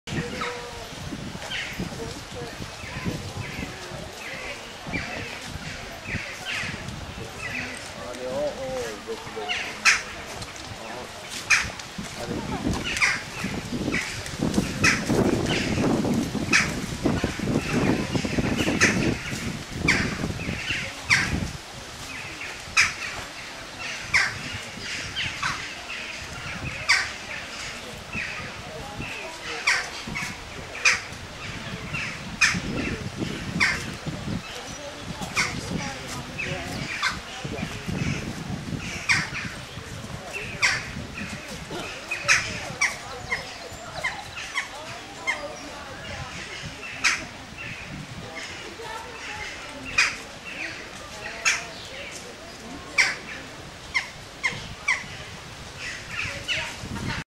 Baby Spider Monkey at Chester Zoo
A baby Colombian spider monkey with its mother in their enclosure at Chester Zoo. Clip shot in July 2018.